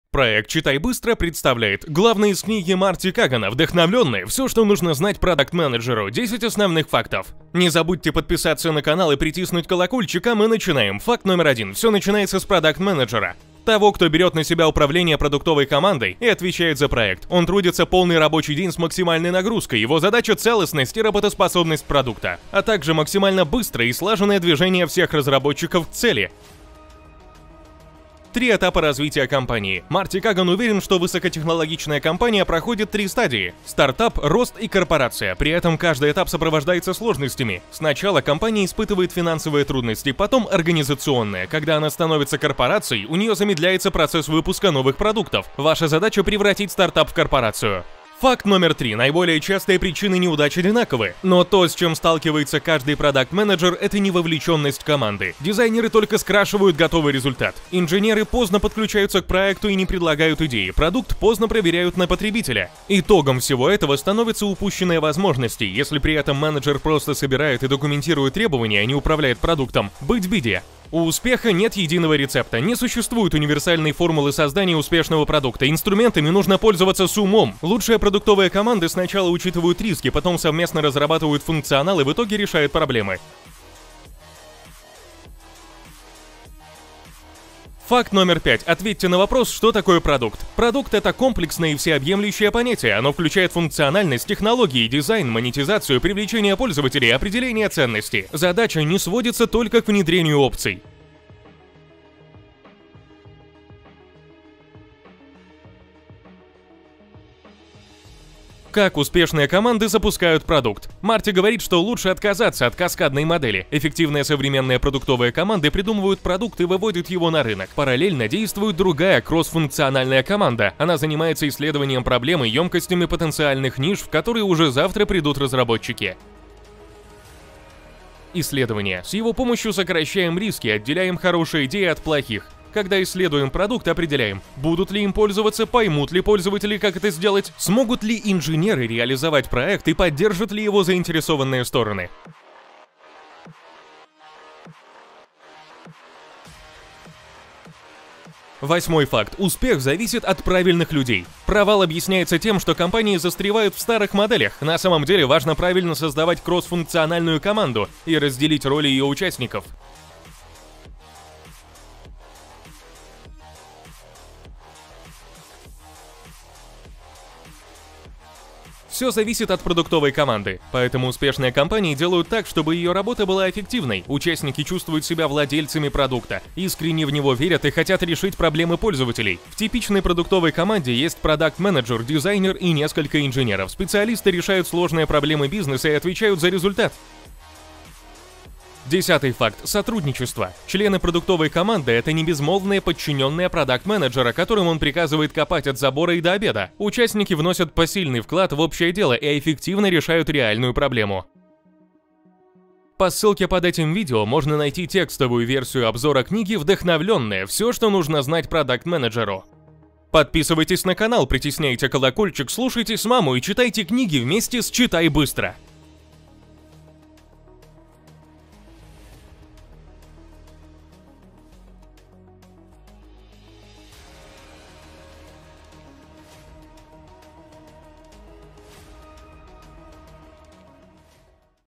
«Вдохновленные», Марти Каган - Книга очень кратко за 3 минуты 20 секунд. Быстрый обзор ⏰
👋 10 фактов из книги Марти Кагана «Вдохновленные. Все, что нужно знать продакт менеджеру» всего за 3 минуты и 20 секунд. А тут текстовая версия

Становись начитанней, пока вдохновляешь разрабов закончить спринт :).

🖤 Если было полезно: лайк, подп... ай, лучше книгу еще одну хорошую почитай и все :)